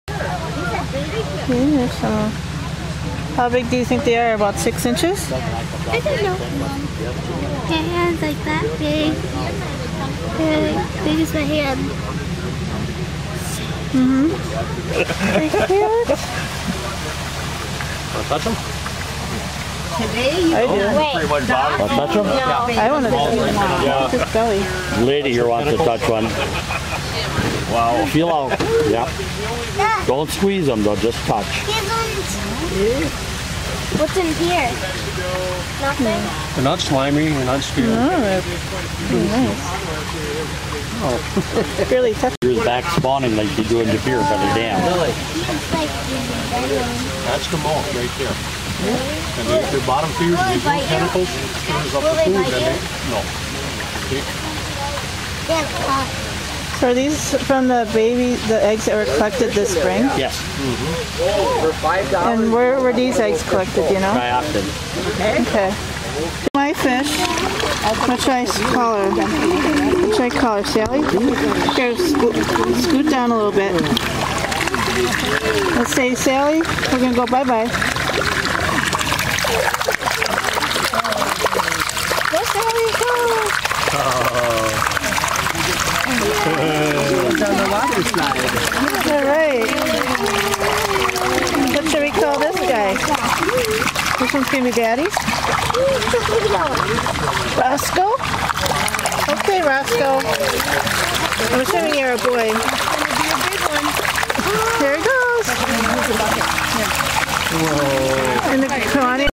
Baby Sturgeon Adoption
These baby sturgeon grew from eggs collected this spring at the Wolf River in Shiocton. Now some of the babies are being released into the Kewaunee River that feeds into Lake Michigan. It is not known yet if these babies will live and spawn in the Kewaunee River of if they will go wherever they want to go. Maybe back to the Wolf River? Maybe we will find out in 12 years when some Sturgeon are ready to spawn. We adopted two fish Sally and Roscoe and we have their tag numbers to look up way down the road.